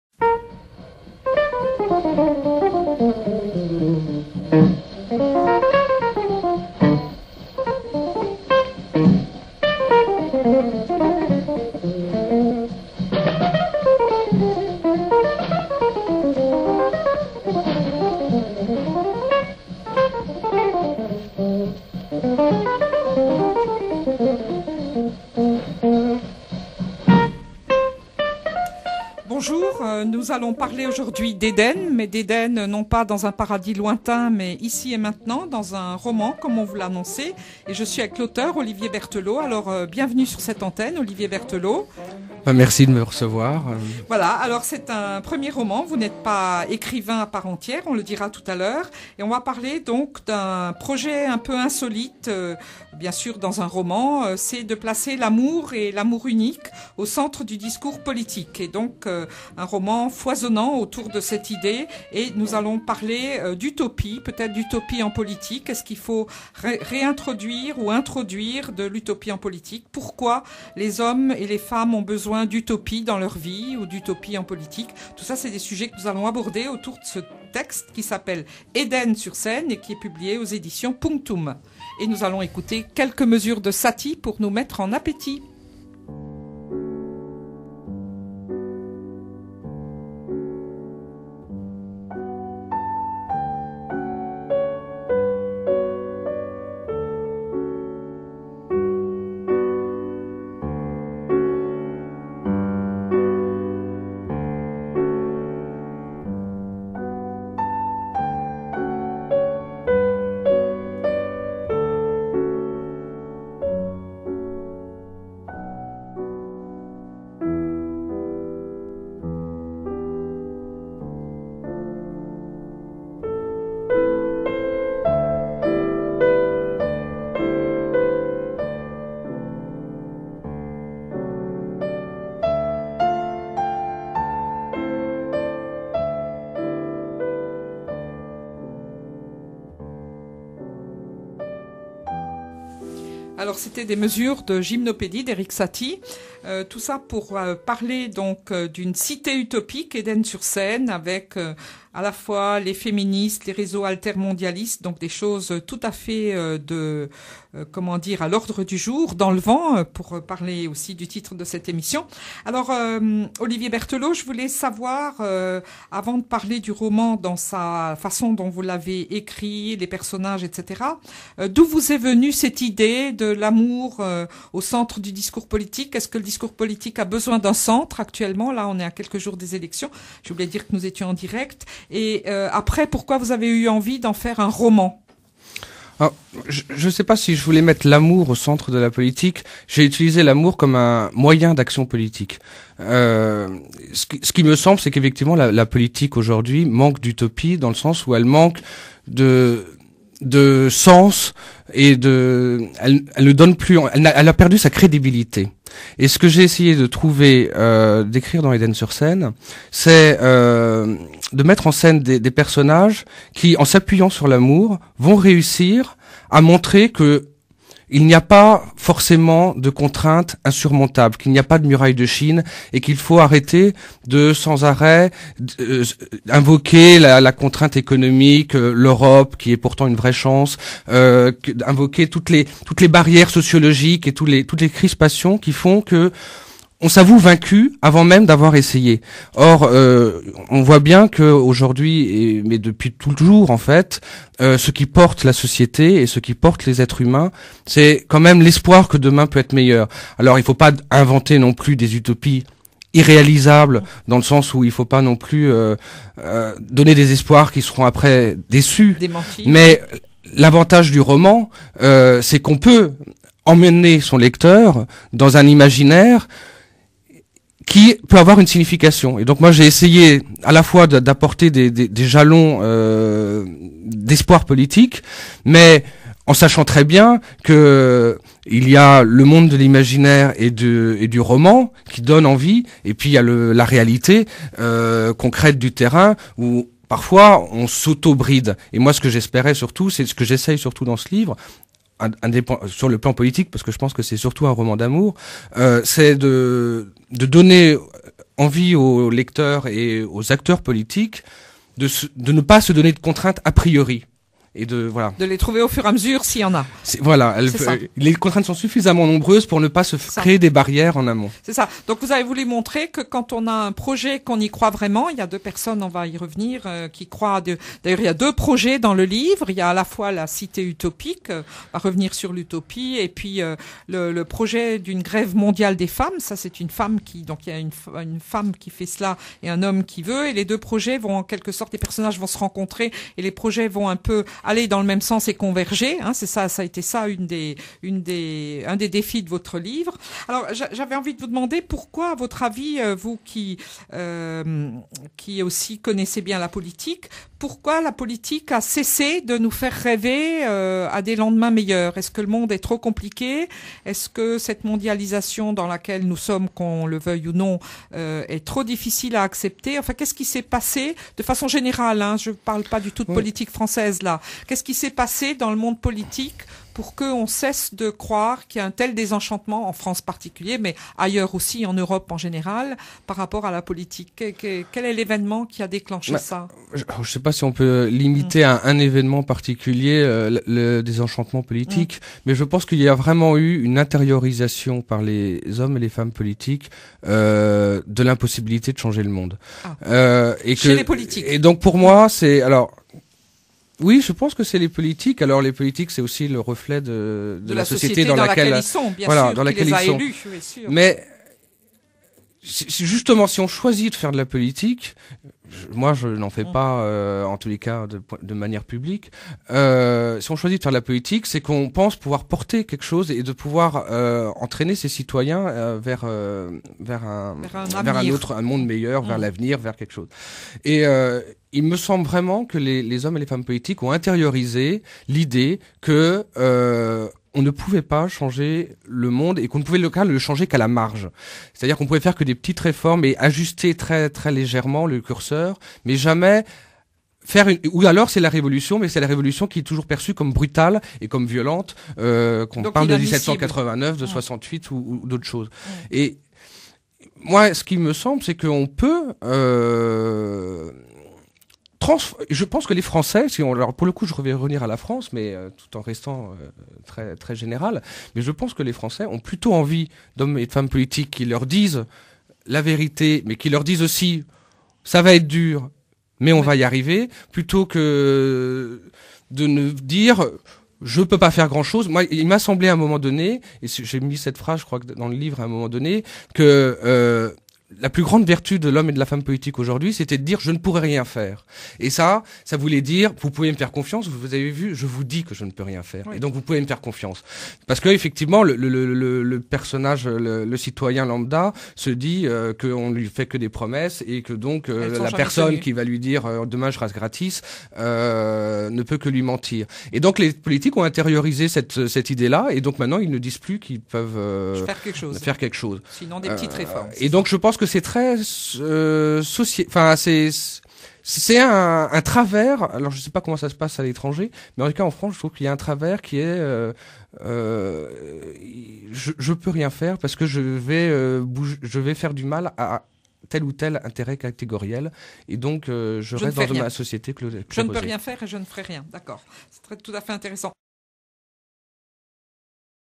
Eden-sur-Seine au fil des ondes
Interview sur Fréquence Protestante (Marie Louise Bernasconi) de Olivier Berthelot sur le roman Eden-sur-Seine